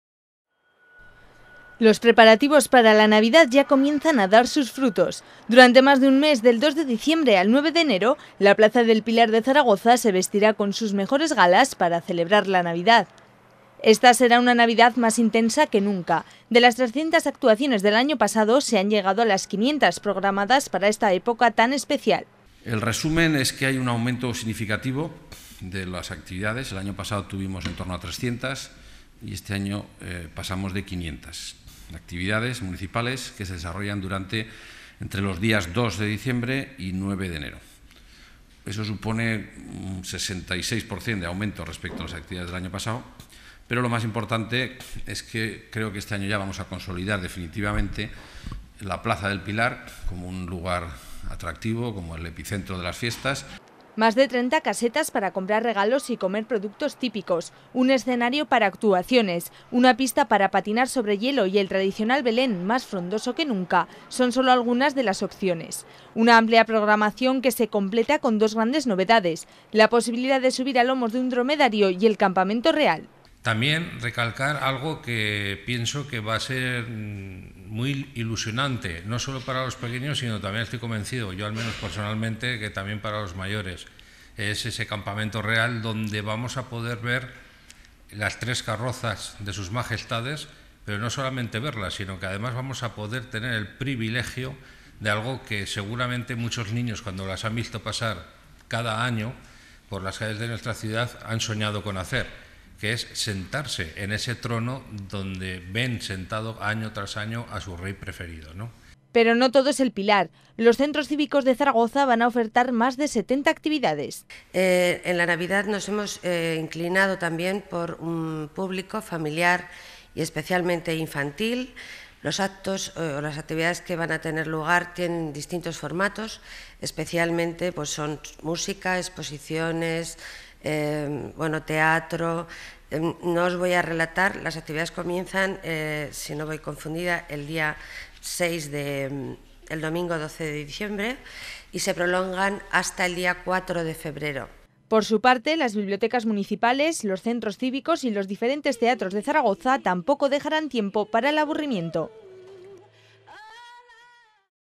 Dromedarios, casetas y el tradicional Belén llenarán la plaza del Pilar en Navidad
(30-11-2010). La plaza del Pilar de Zaragoza se erige como el centro neurálgico de la Navidad. Los zaragozanos podrán hacer casi de todo: patinar sobre hielo, comprar y comer productos típicos en más de 30 casetas y, como novedad, pasear en dromedario. El Belén centrará la mayor parte de las miradas con una escenografía más frondosa que nunca.